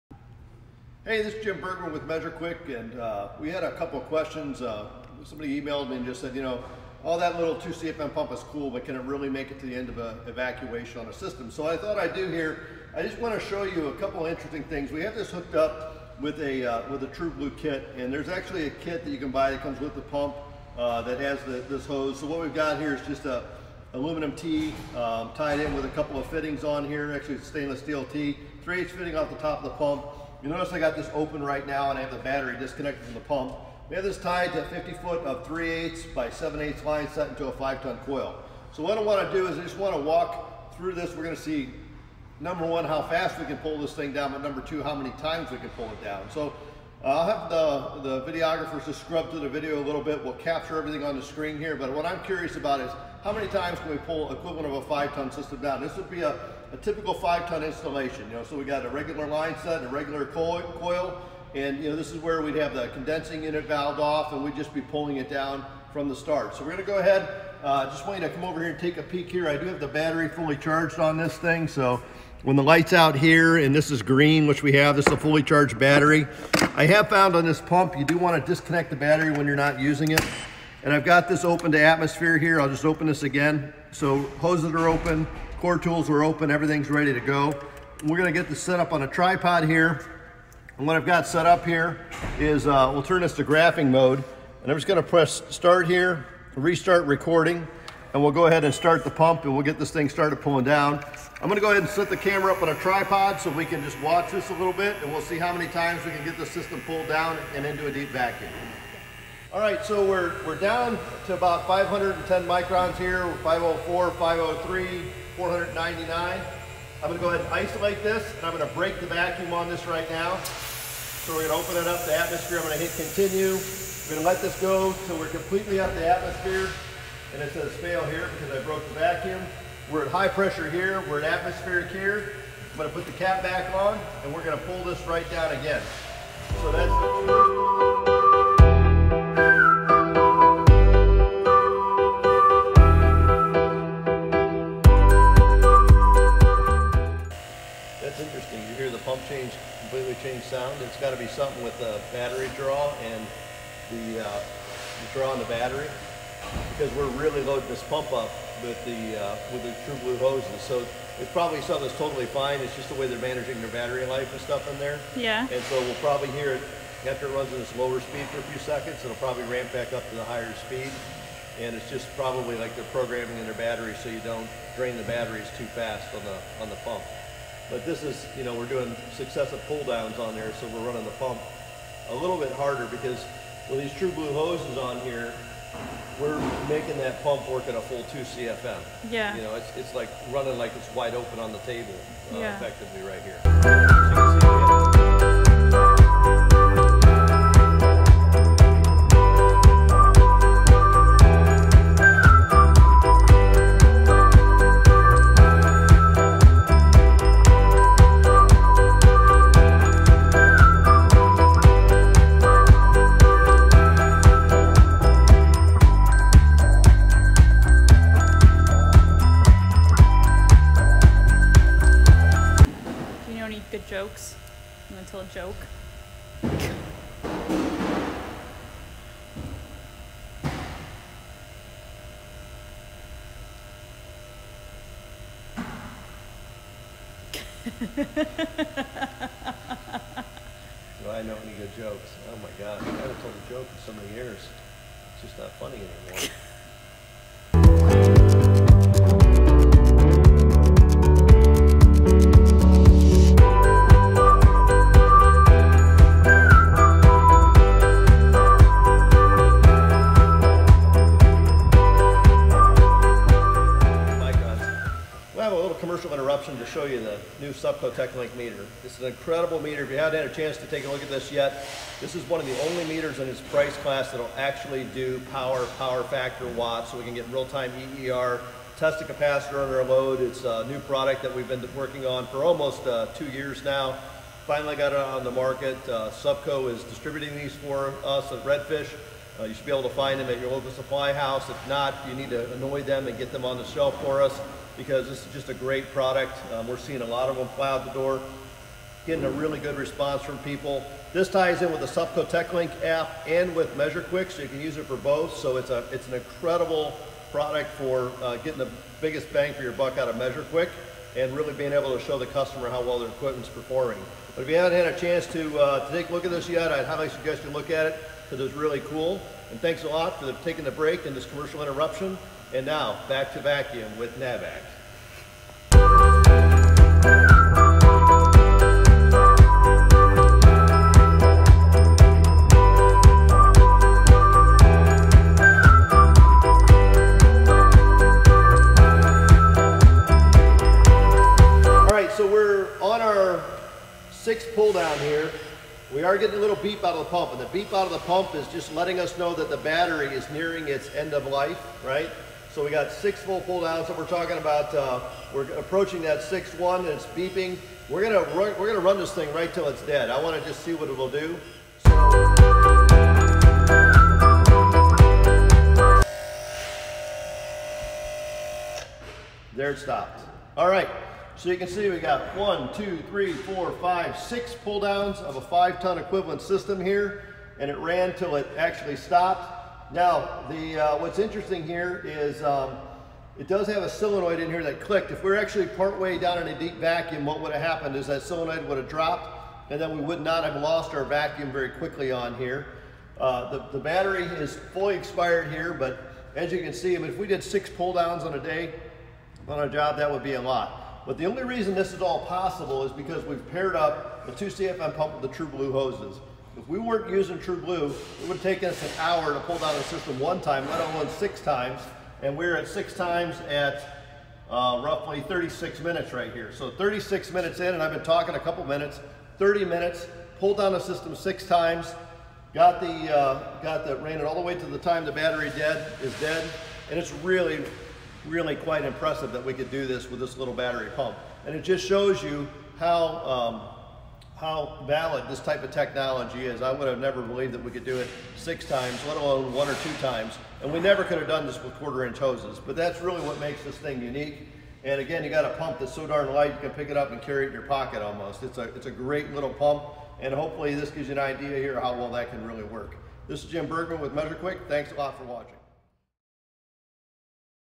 2CFM Navac pump
Jim is testing the 2CFM Navac vacuum pump to its limit.  Let's see how it does.